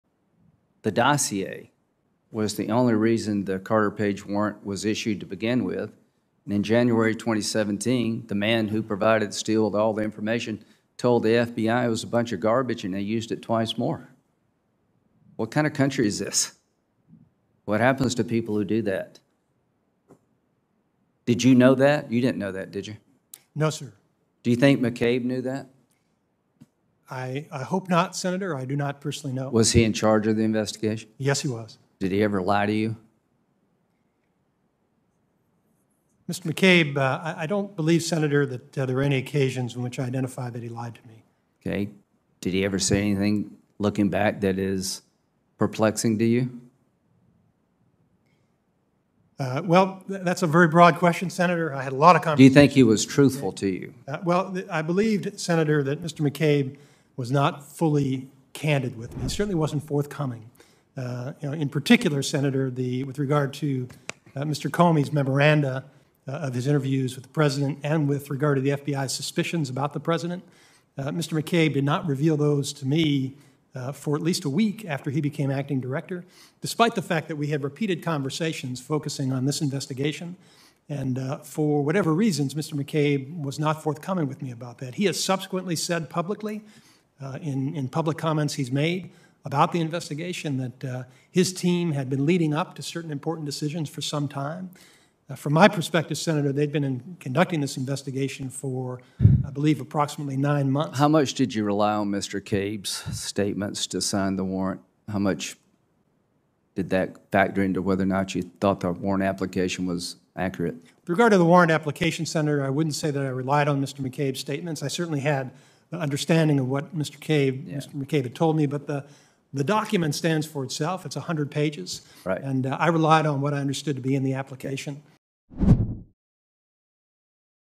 According to Rosenstein, McCabe was not candid with him
Senator Lindsey Graham asked former Deputy Director Rosenstein, if former F.B.I. Deputy Director, McCabe, had lied to him.

From Oversight of the Crossfire Hurricane Investigation: Day 1, June 3, 2020.